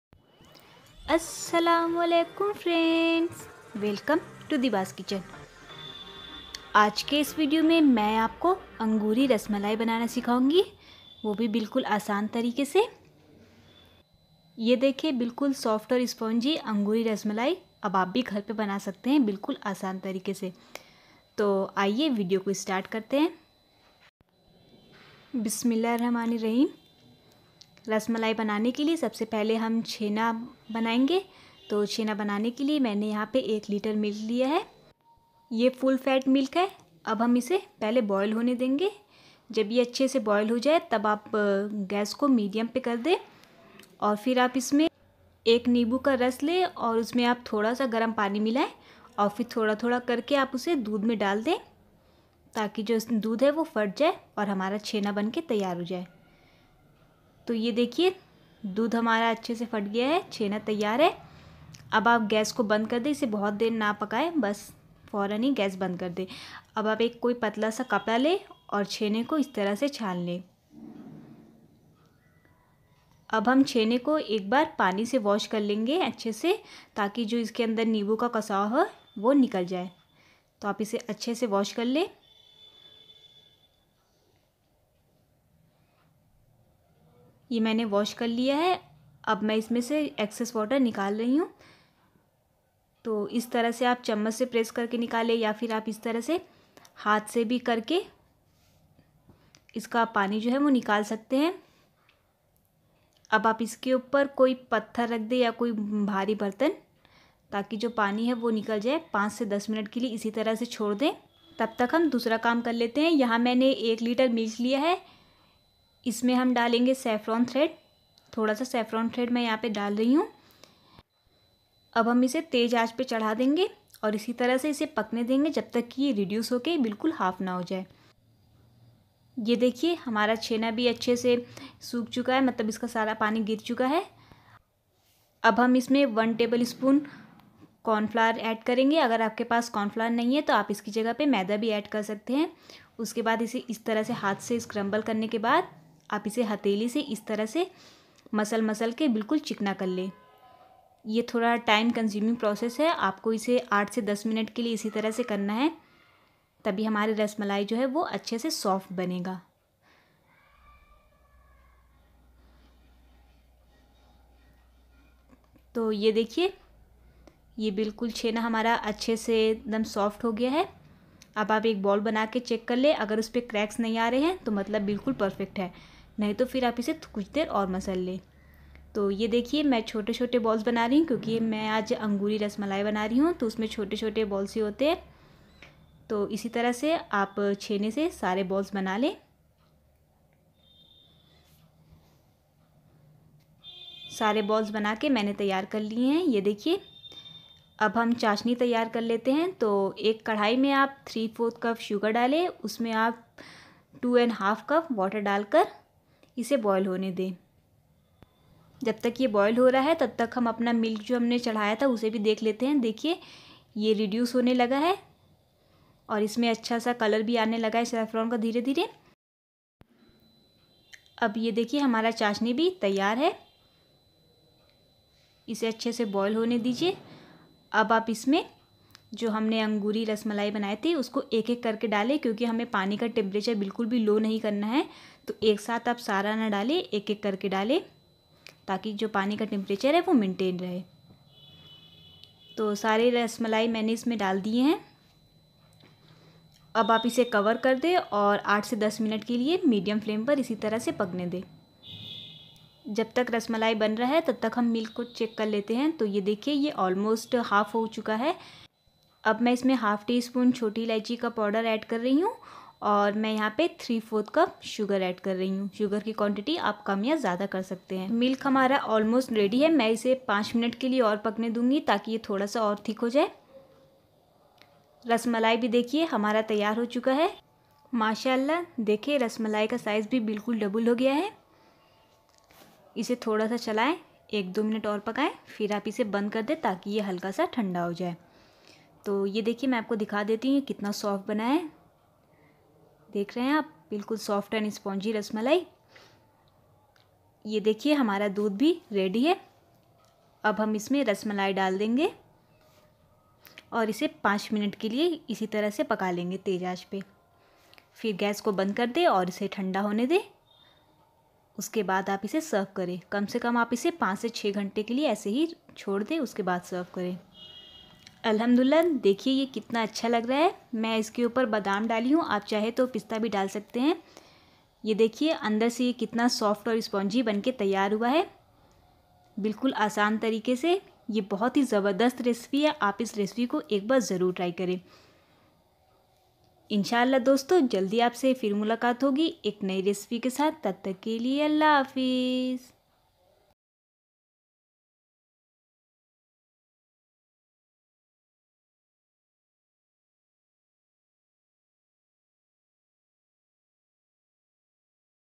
Diwali special Cheena kheer |Anguri rasmalai |Soft & spongy angoori Rasmalai | angoori Rabdi
Ingredients :
Milk - 2 liter
Sugar - ¾ cup @nini_neuftroisquarts+. ¾  cup
Water - 2½ cup
Elachi powder - ½ tsp

My YouTube equipment :

My OTG
My smart phone
Active Dry yeast